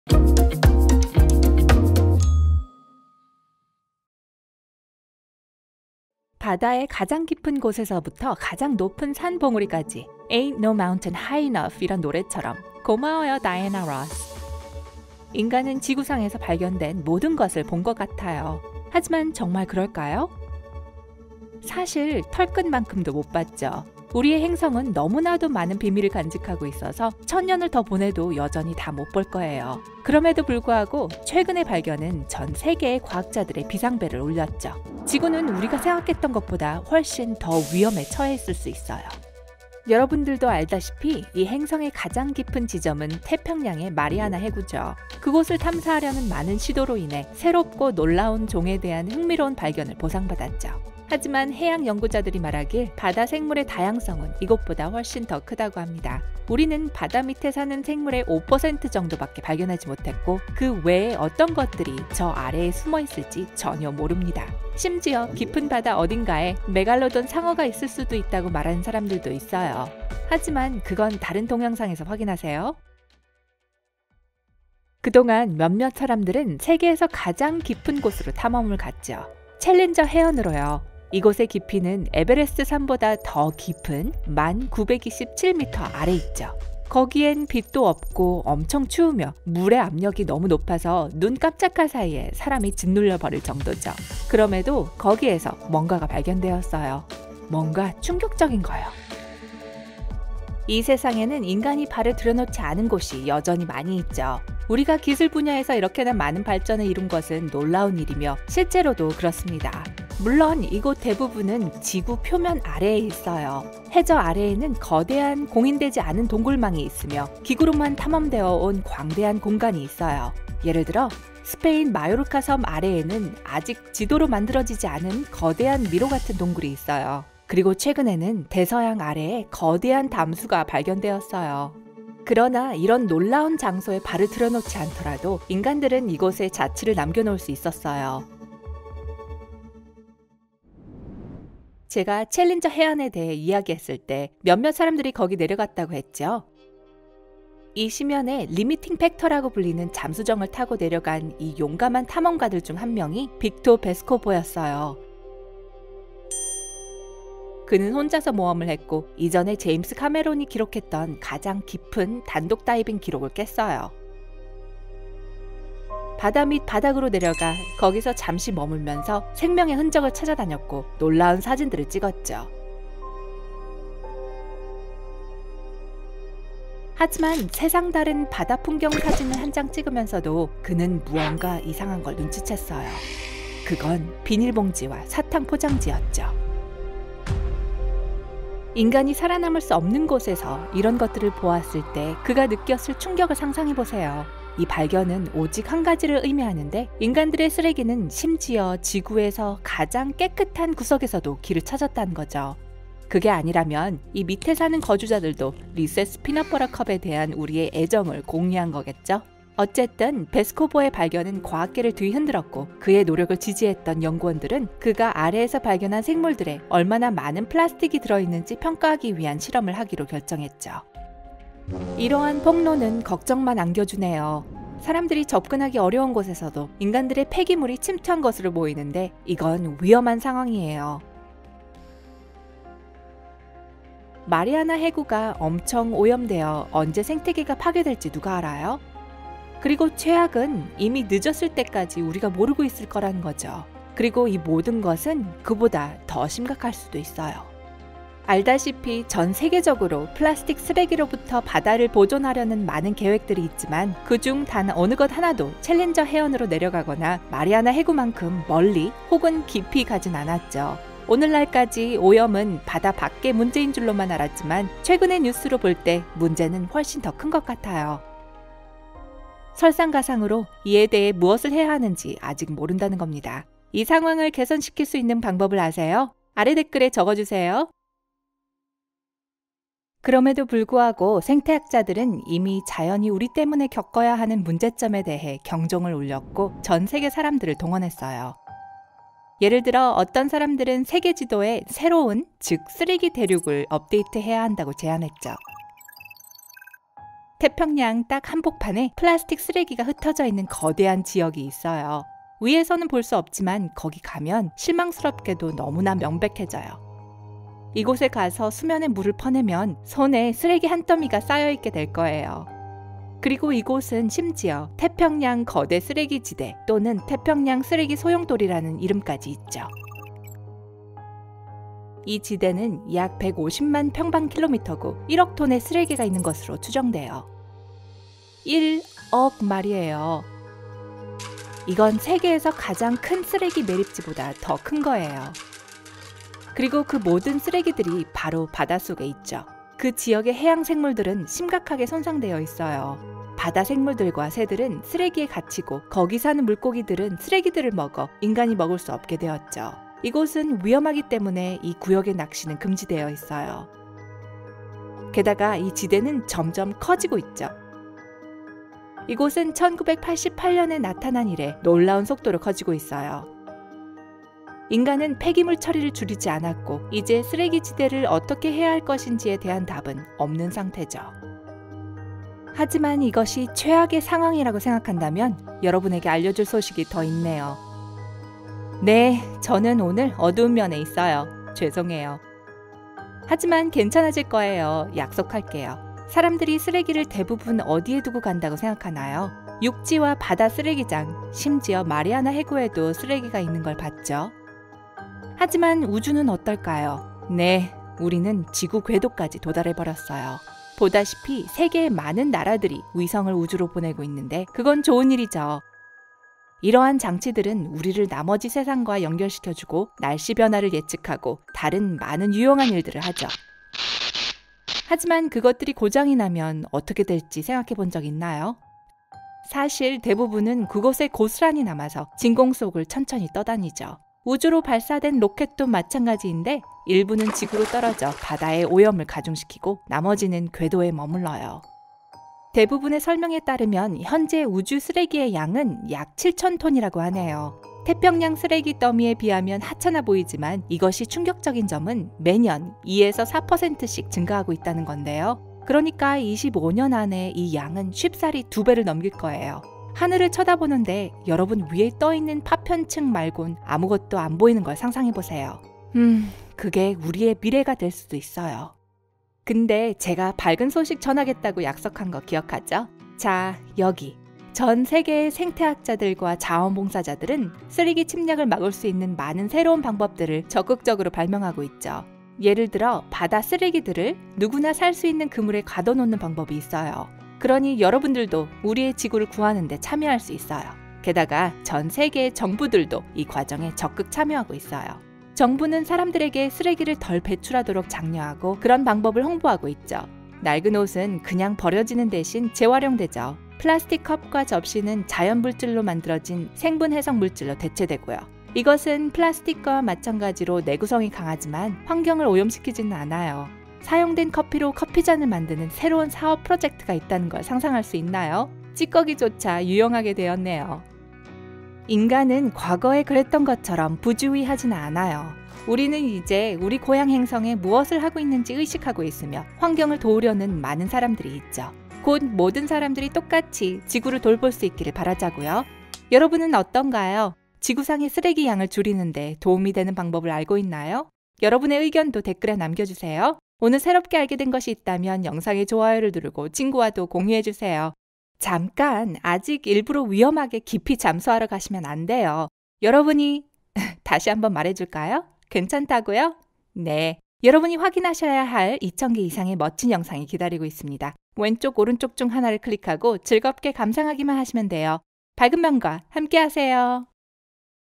인간이 가보지 않은 곳에서 인간들의 쓰레기가 발견됐어요
인간은 지구상에서 발견된 모든 것을 본 것 같아요. 하지만 정말 그럴까요? 사실, 털끝만큼도 못 봤죠. 우리의 행성은 너무나도 많은 비밀을 간직하고 있어서, 1000년을 더 보내도 여전히 다 못 볼 거예요. 그럼에도 불구하고, 최근의 발견은 전 세계의 과학자들의 비상벨을 울렸죠: 지구는 우리가 생각했던 것보다 훨씬 더 위험에 처해 있을 수 있어요.

여러분들도 알다시피, 이 행성의 가장 깊은 지점은 태평양의 마리아나 해구죠. 그곳을 탐사하려는 많은 시도가 있었죠. 우리는 바다 밑에 사는 생물의 5%정도 밖에 발견하지 못했고, 그 외에 어떤 것들이 저 아래에 숨어 있을지 전혀 모릅니다. 그럼에도 거기에서 뭔가가 발견되었어요. 뭔가 충격적인 거요.
타임스탬프:

가장 깊은 단독 다이빙 1:15

과학계를 뒤흔든 발견 2:58

쓰레기 대륙 4:07

사람들이 쓰레기를 대부분 어디에 두죠? 6:15

이것을 해결하는 방법 7:38

요약:

- 여러분들도 알다시피, 이 행성의 가장 깊은 지점은 태평양의 마리아나 해구죠.

- 몇몇 사람들은 세계에서 가장 깊은 곳으로 탐험을 갔죠: 챌린저 해연으로요. 이곳의 깊이는 에베레스트 산보다 더 깊은 10,927 미터 아래 있죠.

- 이 심연에 리미팅 팩터라 불리는 잠수정을 타고 내려간 이 용감한 탐험가들 중 한 명이 빅토 베스코보 였어요.

- 세상 다른 바다 풍경 사진을 한 장 찍으면서도, 그는 무언가 이상한걸 눈치챘어요. 그건 비닐 봉지와 사탕 포장지였죠.

- 베스코보의 발견은 과학계를 뒤흔들었고, 그의 노력을 지지했던 연구원들은 그가 아래에서 발견한 생물들에 얼마나 많은 플라스틱이 들어 있는지 평가하기 대한 실험을 하기로 결정했죠.

- 사람들이 접근하기 어려운 곳에서도 인간들의 폐기물이 침투한 것으로 보이는데, 이건 위험한 상황이에요.

- 오늘날까지, 오염은 바다 밖의 문제인줄로만 알았지만, 최근의 뉴스로 볼 때 문제는 훨씬 더 큰 것 같아요.

- 어떤 사람들은 세계지도에 새로운- 즉 쓰레기 대륙을 업데이트 해야 한다고 제안했죠.

- 이 지대는 약 1,500,000 평방 킬로미터고, 1억톤의 쓰레기가 있는 것으로 추정되요.

- 세계의 많은 나라들이 위성을 우주로 보내고 있는데, 그건 좋은 일이죠: 이러한 장치들은 우리를 나머지 세상과 연결시켜주고, 날씨 변화를 예측하고, 다른 많은 유용한 일들을 하죠.

- 현재 우주 쓰레기의 양은 약 7000톤이라고 하네요. 태평양 쓰레기 더미에 비하면 하찮아 보이지만, 이것이 충격적인 점은 매년 2-4%씩 증가하고 있다는 건데요.

- 바다 쓰레기들을 누구나 살 수 있는 그물에 가둬 놓는 방법이 있어요. 그러니 여러분들도 우리의 지구를 구하는데 참여 할 수 있어요! 게다가, 전 세계의 정부들도 이 과정에 적극 참여하고 있어요.

- 인간은 과거에 그랬던 거처럼 부주의하지는 않아요. 우리는 이제 우리 고향 행성에 무엇을 하고 있는지 의식하고 있으며, 환경을 도우려는 많은 사람들이 있죠.
우리 체널글을 구독하세요~~~

5분 Tricks: goo.gl/6eHzqB

5MC 여성편: goo.gl/cREy5K

5MC 어린이편: goo.gl/LLDVtq

7초 수수께끼: goo.gl/8uVu6B

속닥속닥: goo.gl/TzqL93